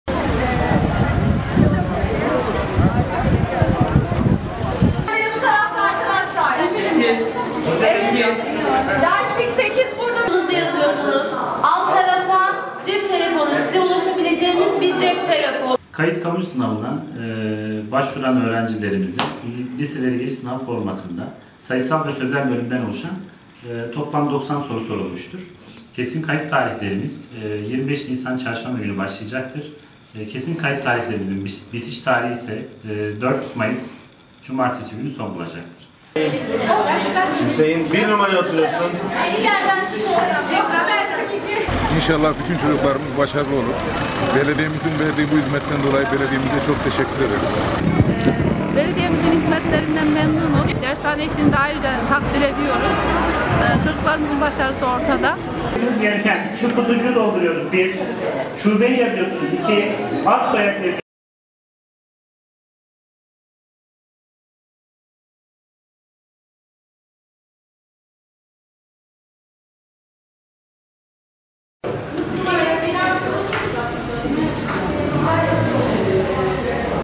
Kurs Merkezi Kayıt Sınavına Büyük İlgi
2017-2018 yılında hizmet vermeye başlayan Erdemli Belediyesi Lise Hazırlık Kurs Merkezi, yeni dönem kayıtları için yaptığı sınava öğrenciler büyük ilgi gösterdiği bildirildi.